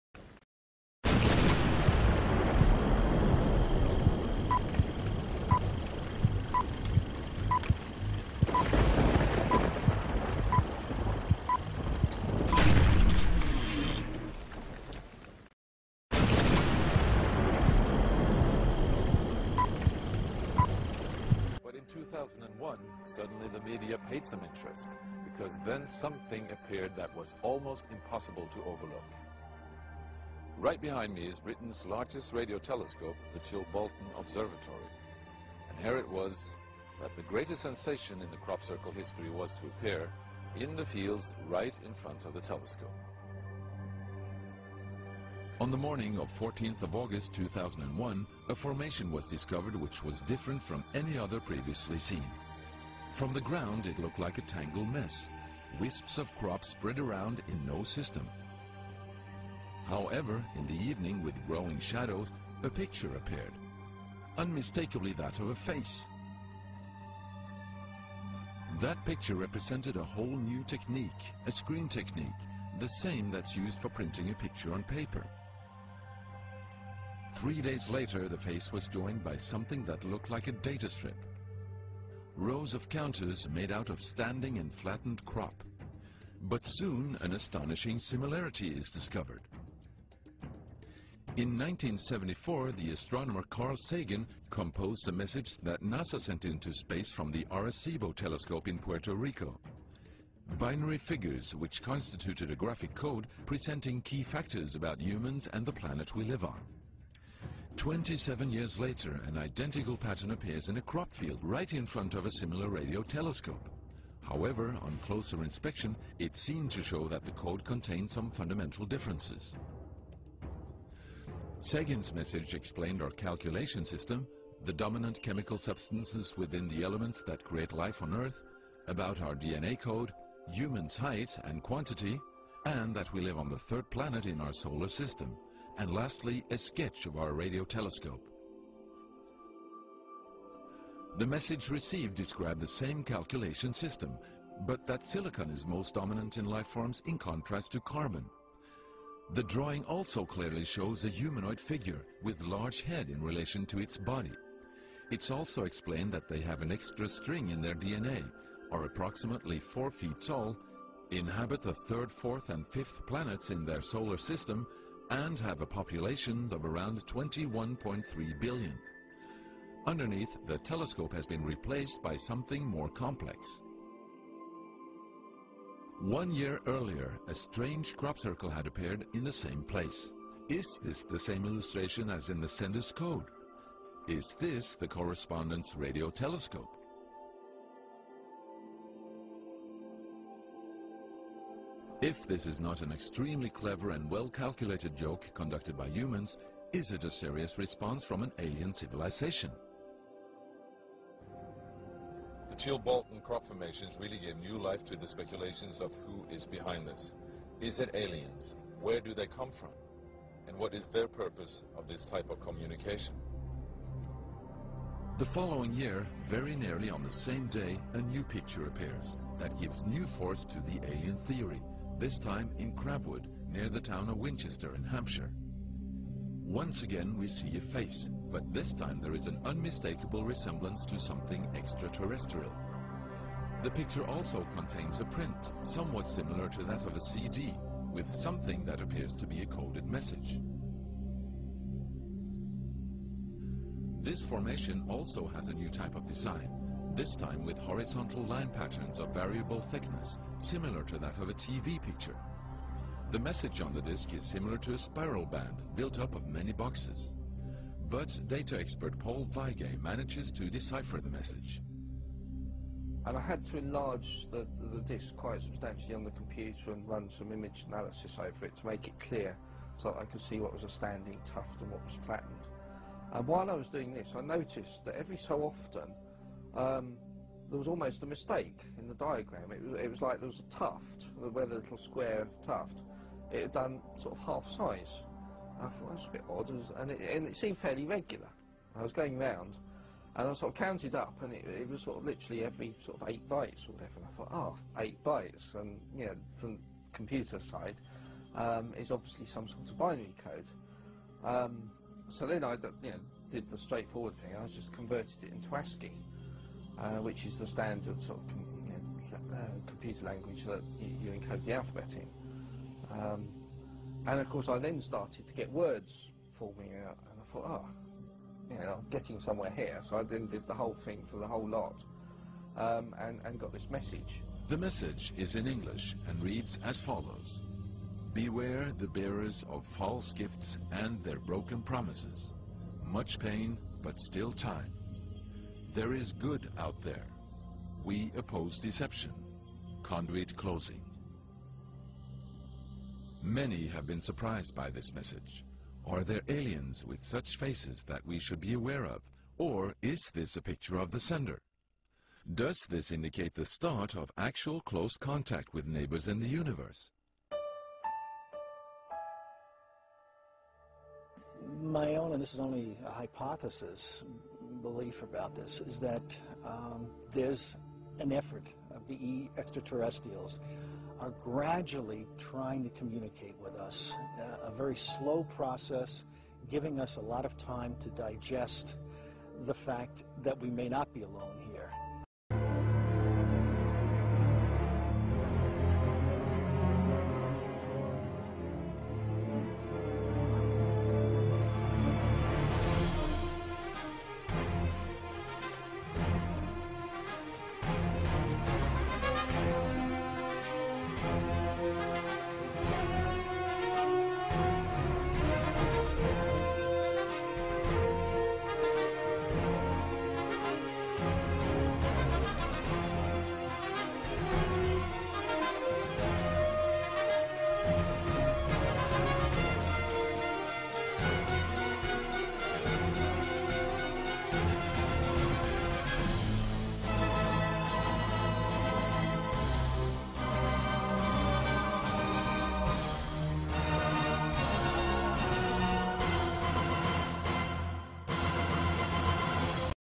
Antwoord van een buitenaardse beschaving met een graancirkel !
In 1974 stuurde de radiotelescoop van Arecibo een FM-boodschap de ruimte in richting het sterrenbeeld Hercules, in de richting van een groep sterren die op 25 000 lichtjaar afstand van de aarde staat. De boodschap bestaat uit een matrix van twee priemgetallen: 23 en 73 en bevat primitieve voorstellingen van de mens en DNA. De boodschap bevindt zich in 2011 op 36 lichtjaar afstand. Tegen de tijd dat de radiosignalen de sterren bereiken, bestaat Arecibo niet meer en zal deze eeuw vervaagd zijn in de herinnering -- als de mensheid dan nog bestaat. De vraag is of iemand of iets ooit het signaal zal opvangen.
In 2001 in Chilbolton, verscheen plots een afbeelding in een granenveld. Voor de wetenschappers was het in eerste instantie niet duidelijk uit te maken wat er nu werd afgebeeld. Totdat men vanuit de lucht nog eens een blik wierp op de afbeelding, en toen duidelijk een menselijk gezicht kon ontwaren.
Drie dagen later verscheen er een tweede afbeelding in het granenveld, schuin naast de eerste van het menselijk hoofd. Ook nu hadden wetenschappers niet door wat er nu werkelijk afgebeeld werd, totdat men de lucht in ging. En tot ieders grote verbazing werd daar de Arecibo-boodschap afgebeeld.
De graancirkels die hierop volgden spreken tot ieders verbeelding !